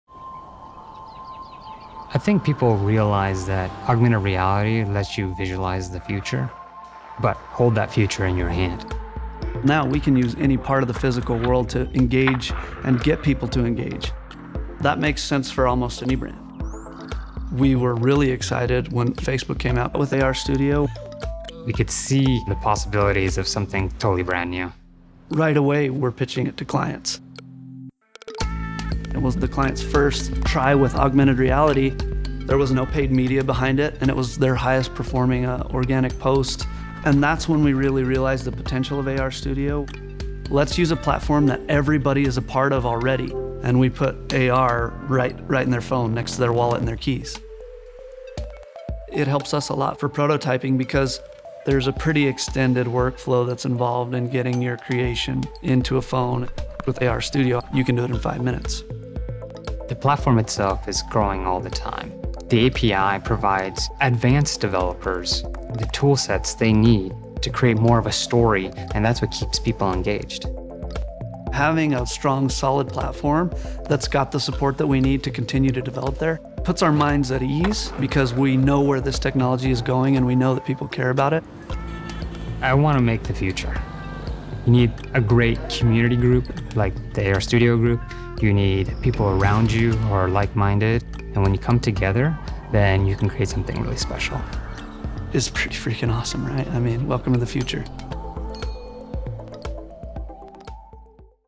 R&R Partners use Spark AR Studio to help brands engage their customers
Augmented reality helps people visualize the future, and hold it in their hand. For R&R Partners, using Facebook’s Spark AR Studio has allowed them to use any part of the physical world to create engaging content for the clients they work with. With Spark AR Studio, they’ve been able to help a variety of brands tell their stories.